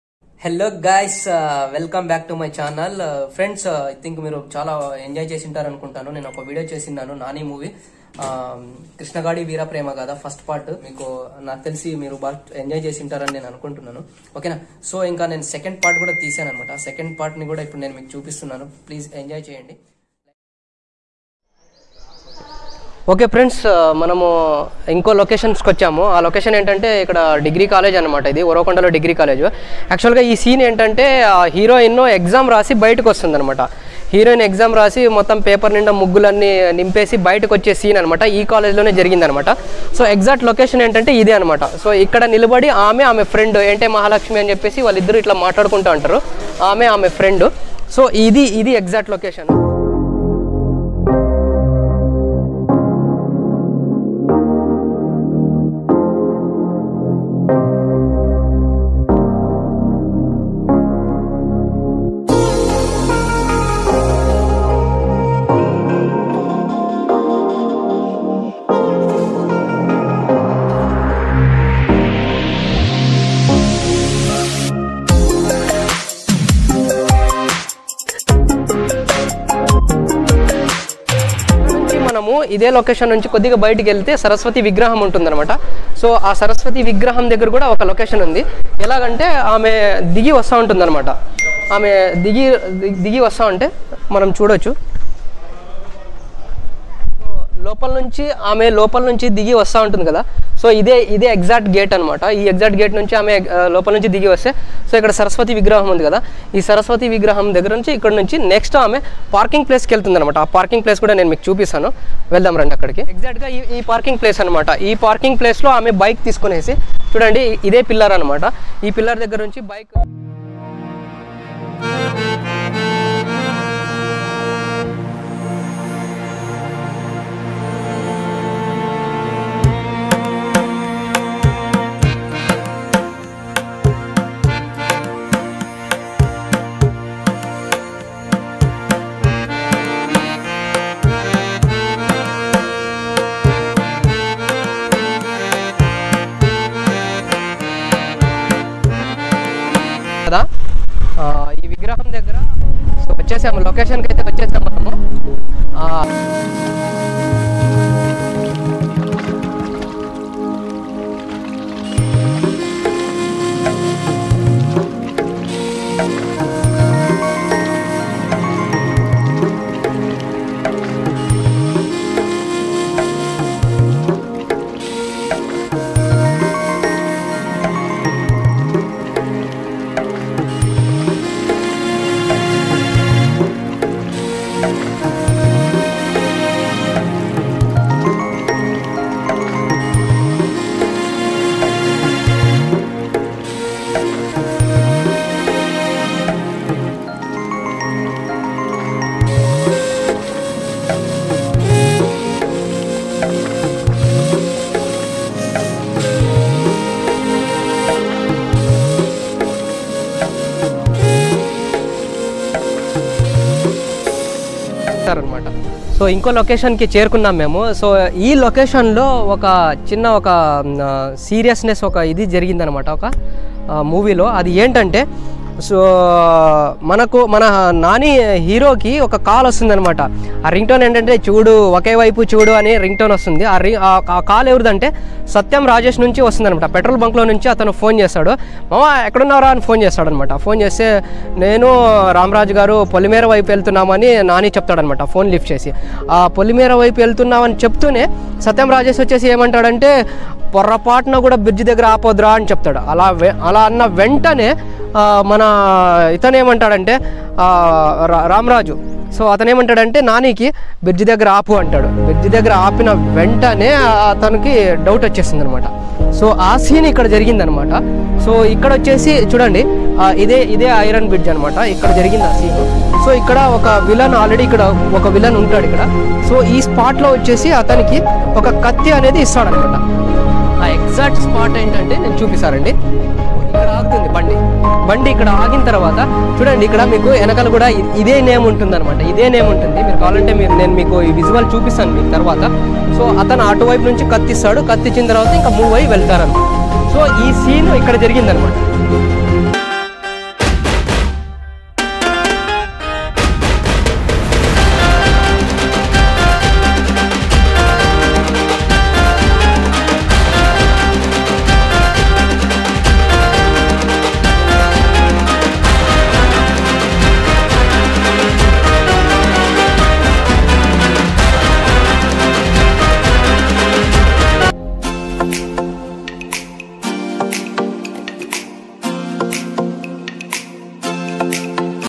Krishna Gaadi Veera Prema Gaadha Movie Locations | Anantapur Uravakonda | Nani Shooting Seens |Part2
Krishna Gaadi Veera Prema Gaadha Movie Locations | Anantapur Uravakonda | Nani Shooting Seens | Part 2

Anantaputr Uravakonda
Nani Movie Locations
# Krishna gaadi veera prema Gaadha

Hi Viewers,
Video regarding Nani Movie Krishna gaadi veera prema gaadha Movie location Seens.
movie was naked in the year 2016 Some of seens are shooted in Anantapur Uravakonda near by location. We have made the some location seens in this video to our viewers to watch and enjoy.

SFR Telugu Vlogs